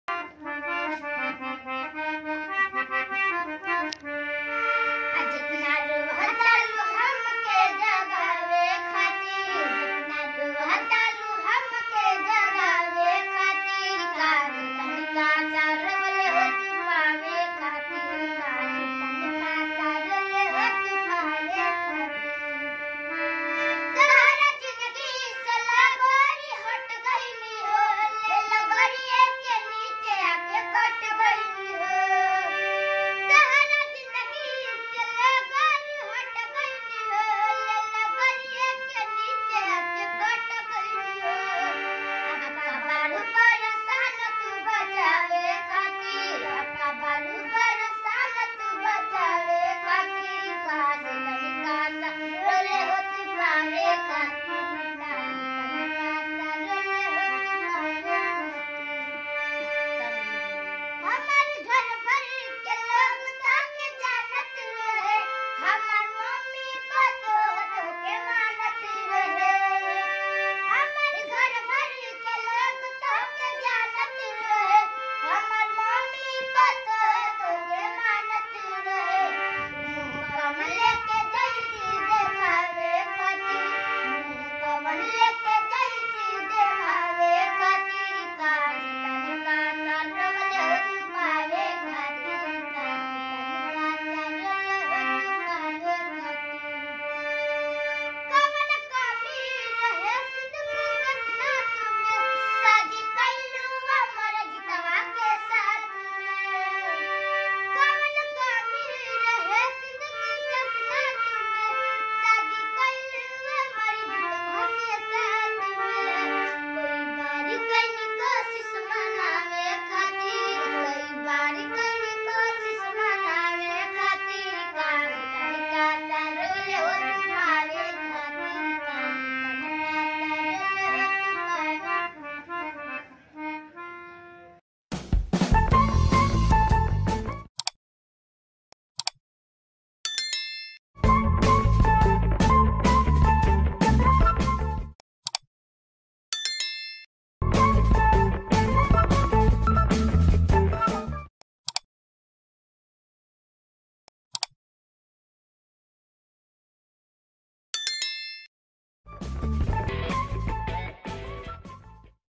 भोजपुरी 🤭दोनों छोटी-छोटी बहनों ने ऐसा क्या गा दीया कैमरा वाला खुद😀 हैरान हो गया वीडियो जरूर देखें
भोजपुरी वीडियो दोनों छोटी छोटी बहनों ने ऐसा क्या गाना गा दिया कि कैमरा वाला खुद हैरान हो गया अगर वीडियो अच्छा लगे तो प्लीज चैनल को सब्सक्राइब और लाइक कमेंट और ज्यादा जगह शेयर जरूर करें धन्यवाद